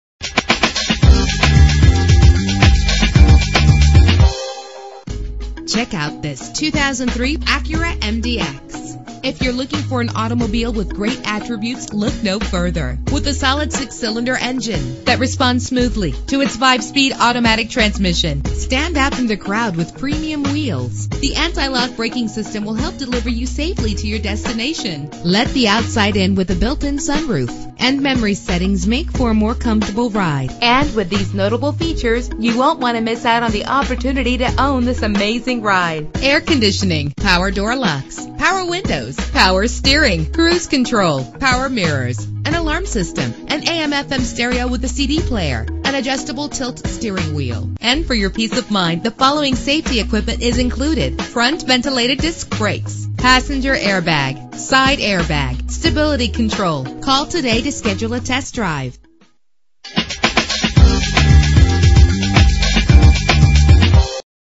2003 Acura MDX - Clearwater FL
Year: 2003
Make: Acura
Model: MDX
Trim: 3.5 Touring w/ RES
Engine: 3.5 liter 6 cylinder 24 valve
Transmission: 5-Speed Automatic
Color: Nighthawk Black Pearl
Mileage: 122733
Address:
18911 US. 19 N.